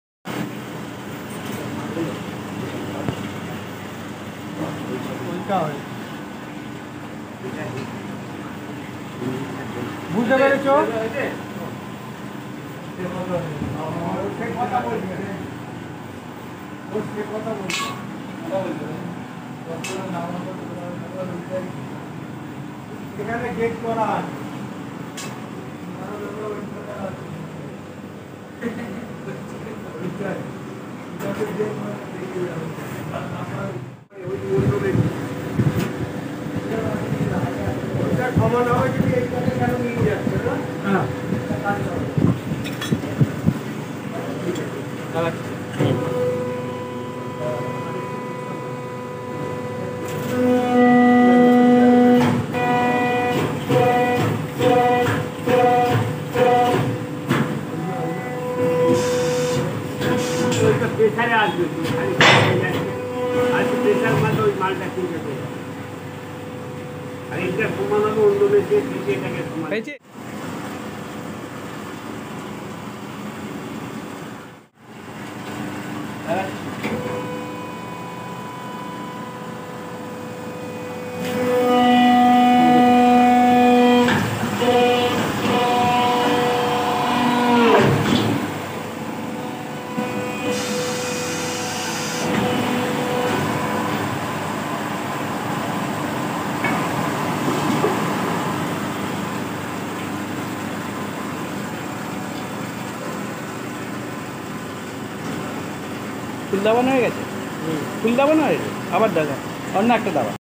hydraulicpress machine making tasla gamla kadai deepdrawingpress by Naskar & company
Naskar & company manufacturer of mohamaya machine we make complete projetcs for sheet metal works hydraulicpress trial run before deliveey for making tasla gamla kadai etc. we setup full project all operation video are comming soon please subscribe us for all updates.
call or whatsaap us on